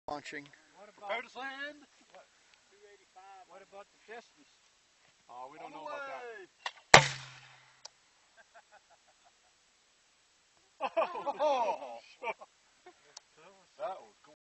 60mm bud light mortar launcher
These aluminum bud light long necks have found a purpose other then being thrown away after consumption. Here we have taken them and filled them with concrete and put a nut in the end. Now we can screw in the normal mortar fins and the bottles fit just perfectly in this 60mm mortar launcher.